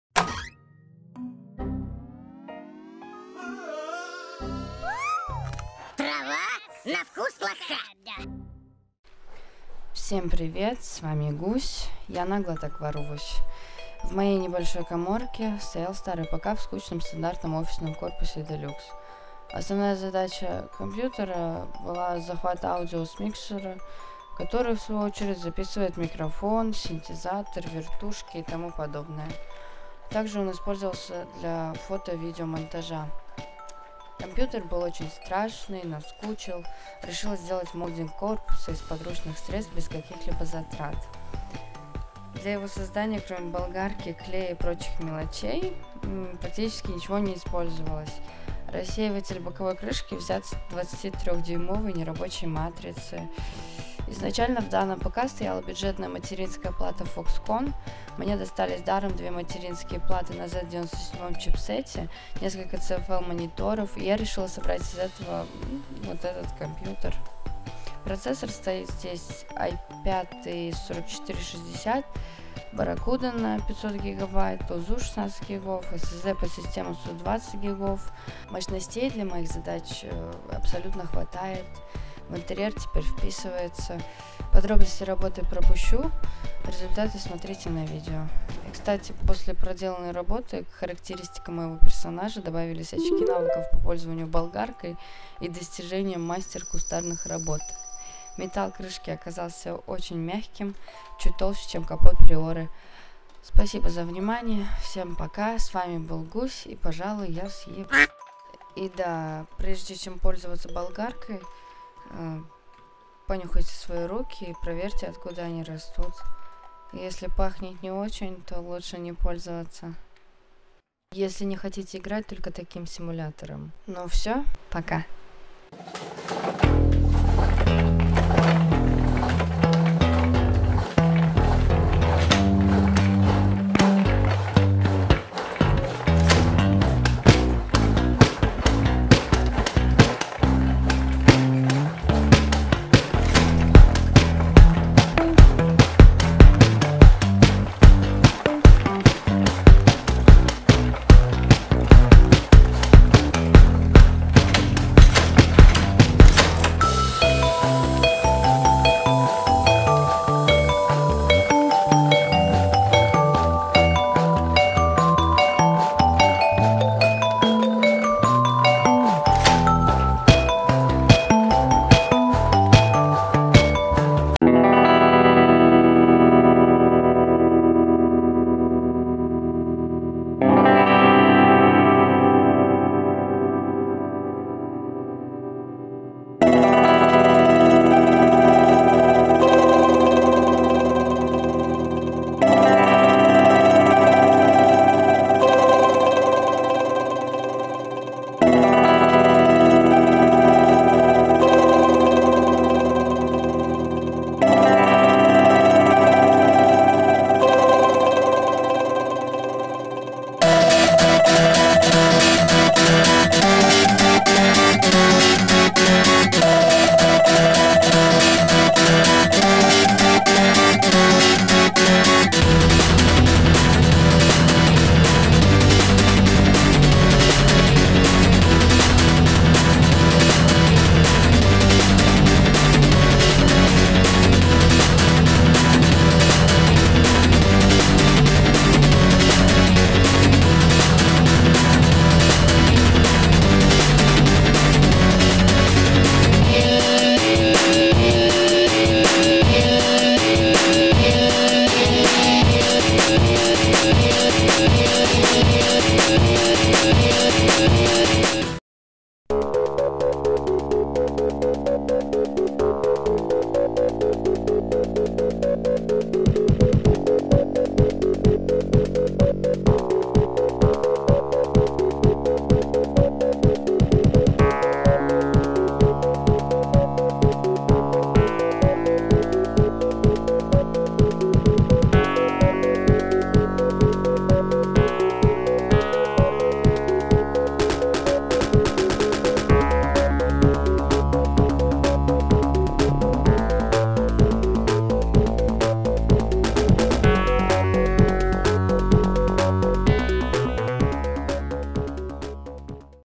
БОМЖ - МОДДИНГ ОФИСНОГО КОРПУСА (ПК)
Привет, я Гусь, и я решила заняться моддингом своего старого компа.
Все подробности в видео. Дополнительная информация: LGA 1150